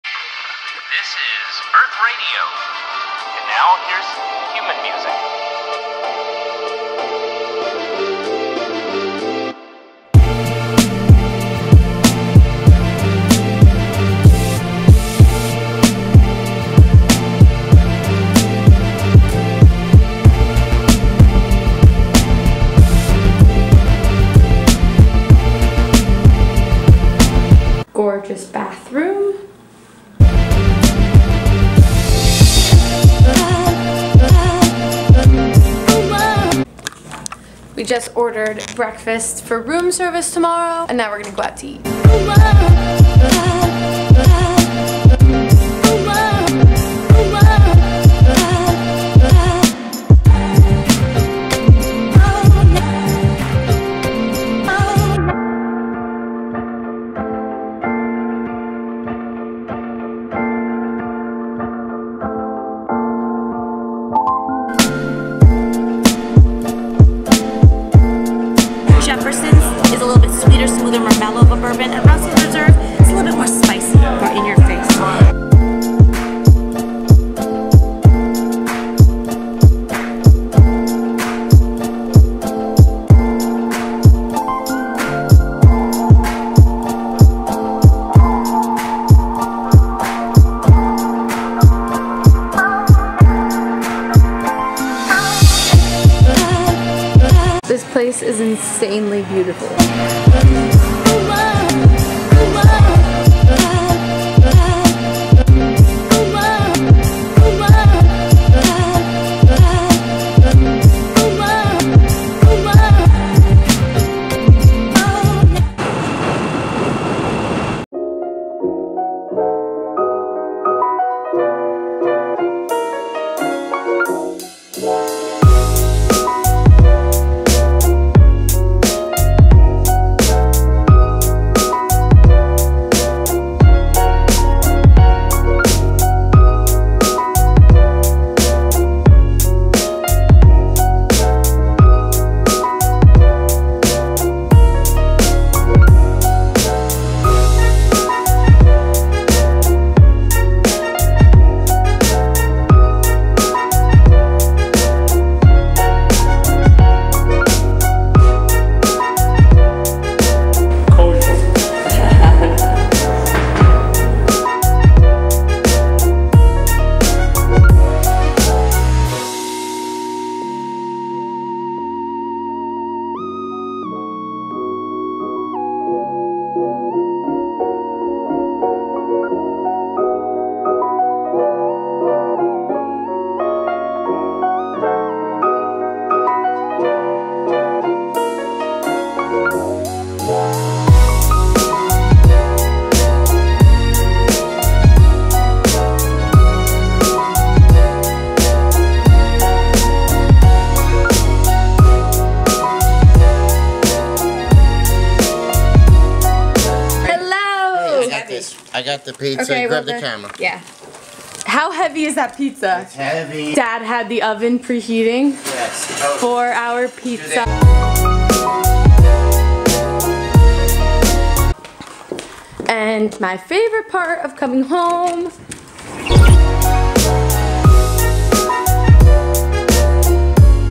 JETSMARTER :: FLYING DEEP DISH PIZZA HOME WITH ME : CHICAGO VLOG
My first time visiting Chicago! I didn't get to try deep dish pizza due to meetings so we ordered three large Lou Malnati's deep dish pizzas, froze them at the hotel (shoutout The Kimpton Gray), then carried them home on the plane with us!